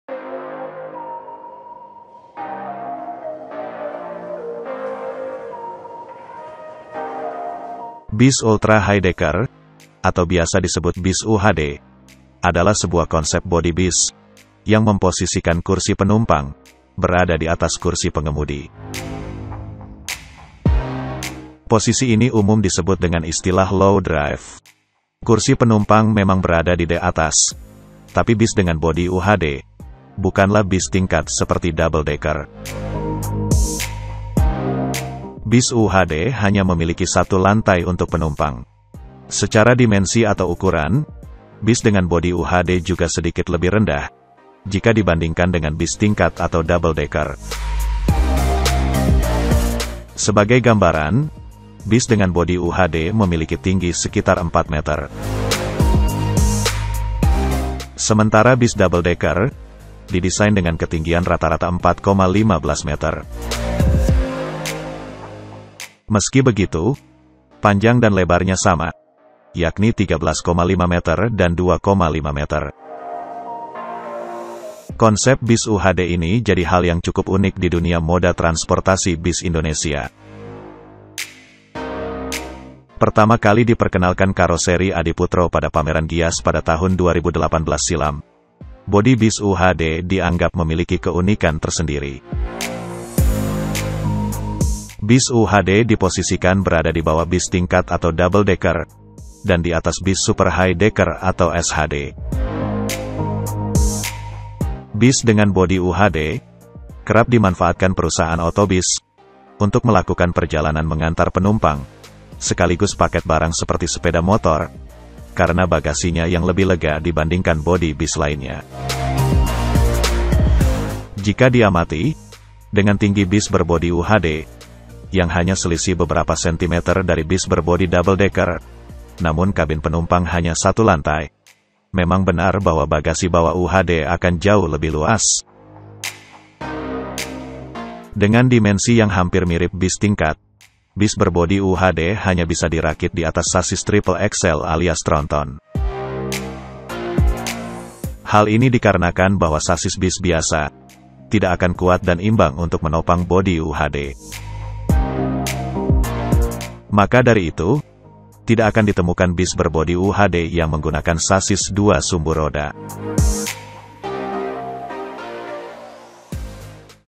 KARENA INI...!!! BUS UHD SELALU TRONTON
TERJAWAB...!!! Mengapa Bus Bodi Ultra High Deck selalu menggunakan Sasis Tronton. Alasan Bus UHD harus menggunakan Sasis Tronton

Dapatkan 80RB langsung pada aplikasi REDBUS dengan menggunakan
KODE REFERRAL REDBUS: redaipbz

SALAM PENGELANA...!!!

Mengapa Bus Bodi Ultra High Deck selalu menggunakan Sasis Tronton. Alasan Bus UHD harus menggunakan Sasis Tronton